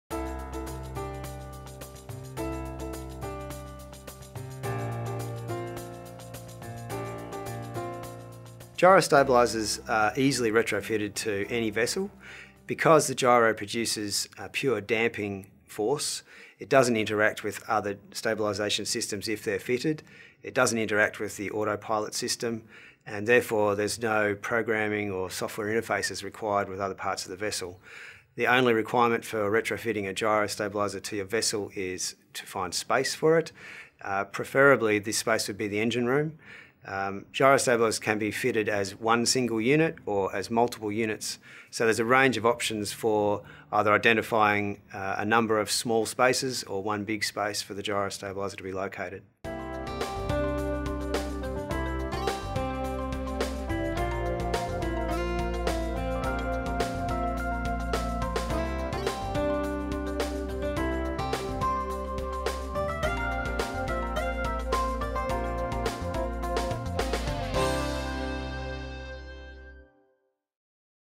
16 Can I retrofit a VEEM Gyro?
Q: Can I retrofit a VEEM Gyro?
A: VEEM Gyros are easily retrofitted to any vessel. Because the gyro produces a pure damping force, it doesn't interact with other stabilisation systems if they are fitted. It doesn't interact with the autopilot system and therefore, there is no programming or software interfaces required with other parts of the vessel. The only requirement for retrofitting a Halcyon Gyrostabiliser to your vessel is to find space for it. Preferably, this space would be the engine room. VEEM Gyros can be fitted as one single unit or as multiple units, so there are a range of options for either identifying a number of small spaces or one big space for the gyrostabiliser to be located.